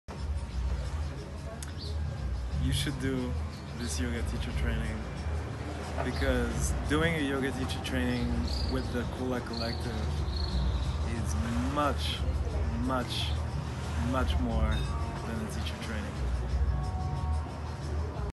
Kula Yoga Teacher Training Love: Arthur Bernard's Story
Hear about how Arthur's Yoga Teacher Training Experience with Kula Collective transformed his life!

Learn more about Kula's 200 hr and 300 hr Yoga Teacher Trainings and how they can impact your spiritual evolution. Explore, Empower and Expand with us!